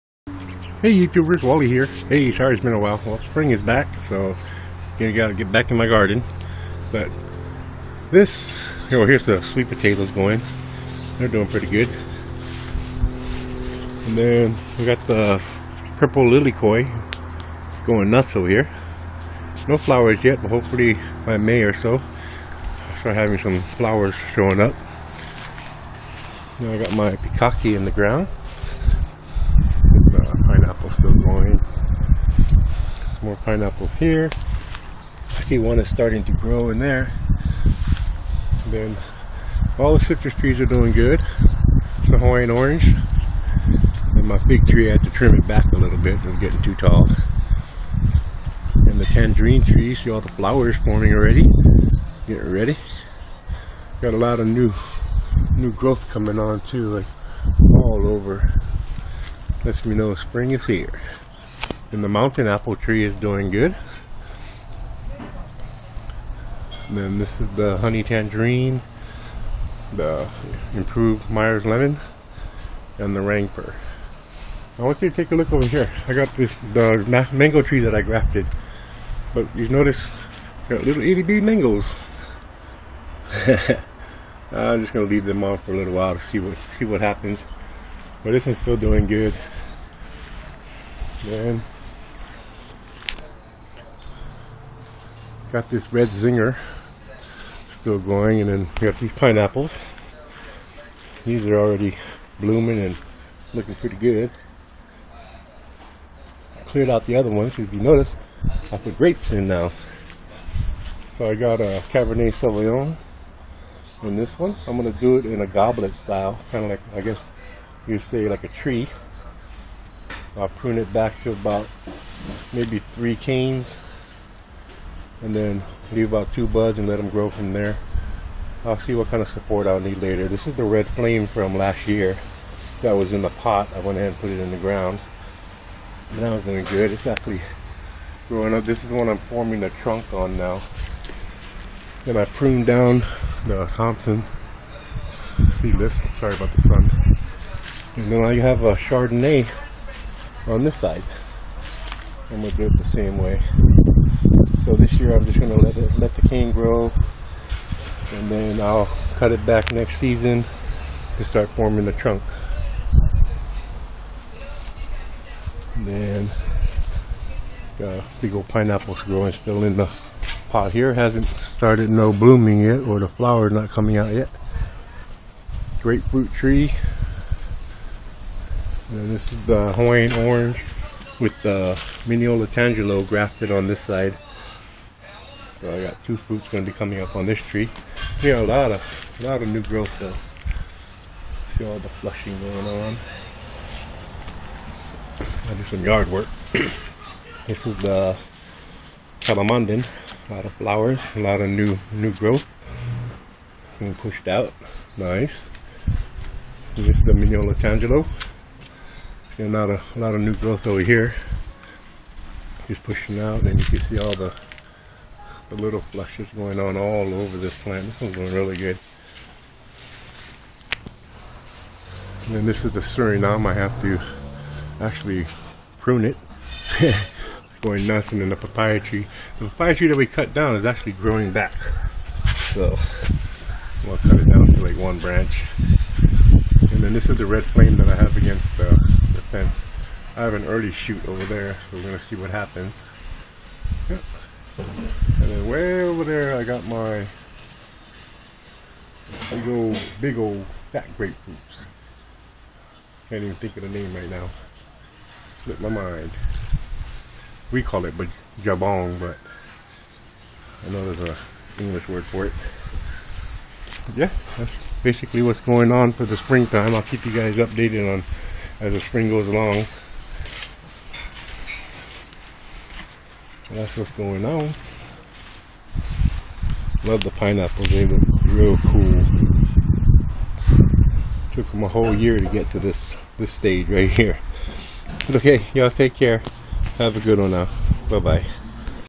YaY!! spring is here, See what's new in my GaRdEn for 2014
I have some new projects going on with some grape plants, table and wine varieties.  Come see what's still growing and get the update on my citrus trees, thanks for coming by have a wonderful spring time